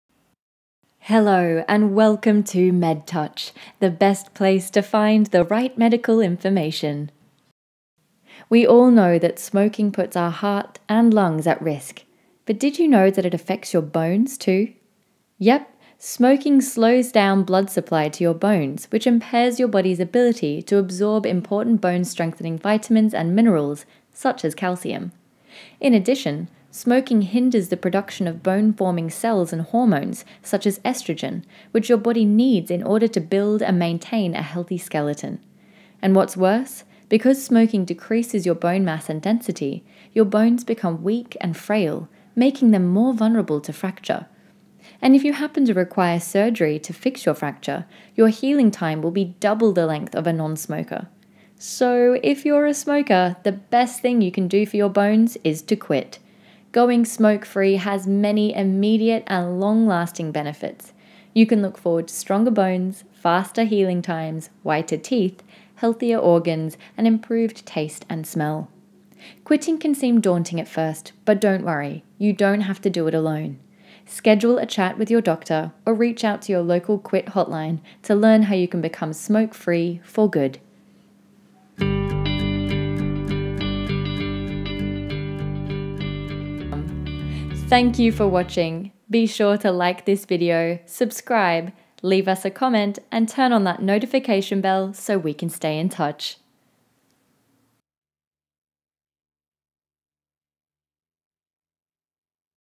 Does smoking affect your bones?
Smoking can be harmful for bone health. Let’s find out why!

💛Your wellness. Our vision 😊

🙌 Thank you for showing your support by watching, liking, commenting, and sharing our videos.